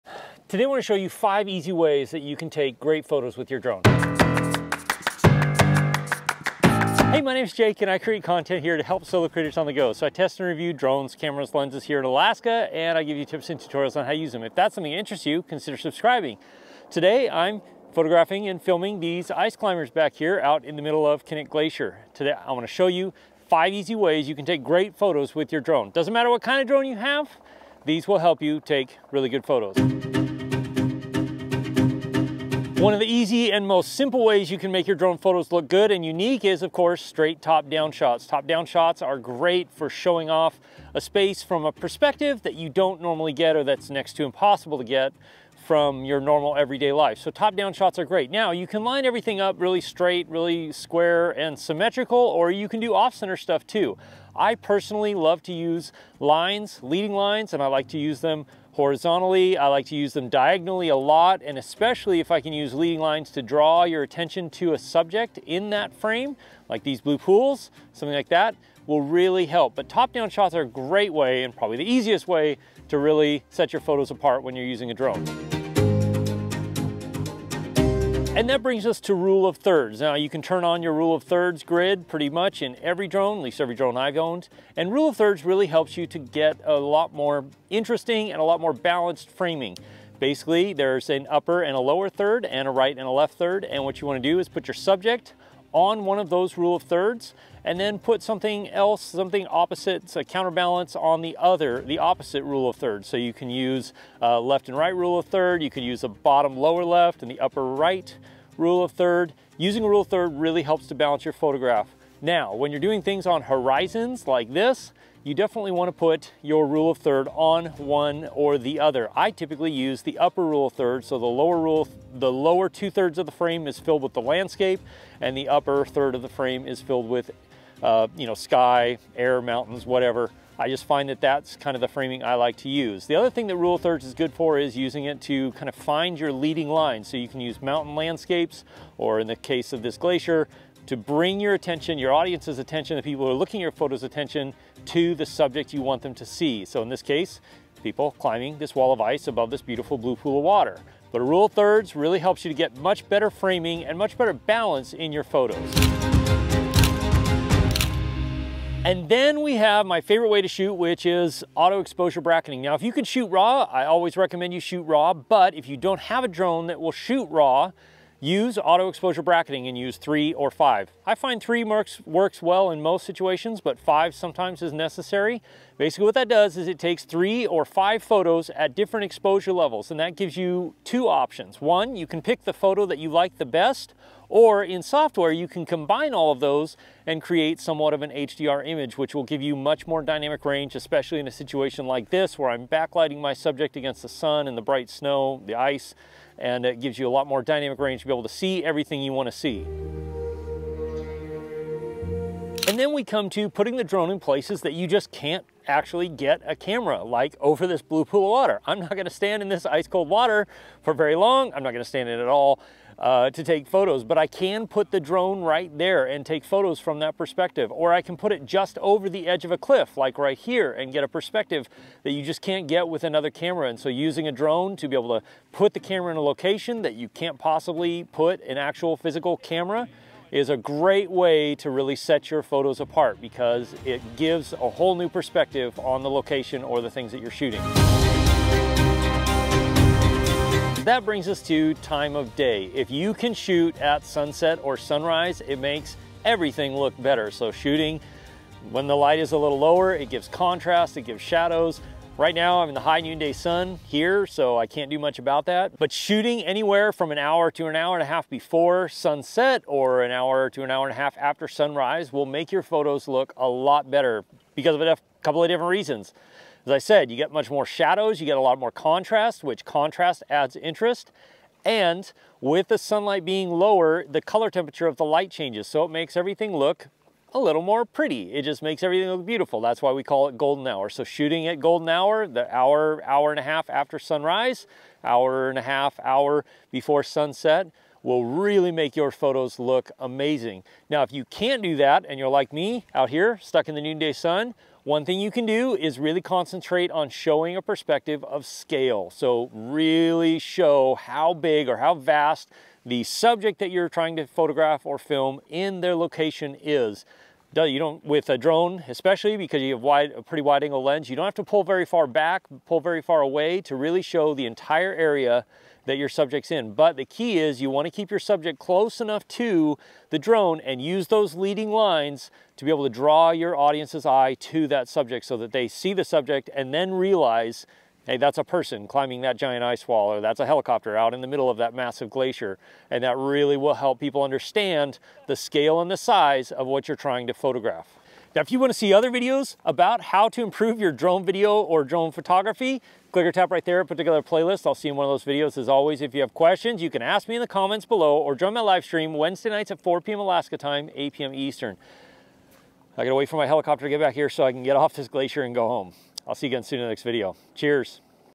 5 Easy Ways to Take Great Photos with Your Drone
Do you want to take great photos with your drone? There are 5 super easy ways you can take great photos with any drone. I'll show you these easy steps to improve your drone photography wether you have a mini 2 mini 3 pro or a larger drone like the mavic 3 these simple steps will help you get great photos from your drone every time.

Learn how to use and fly your drone to create professional results by taking my course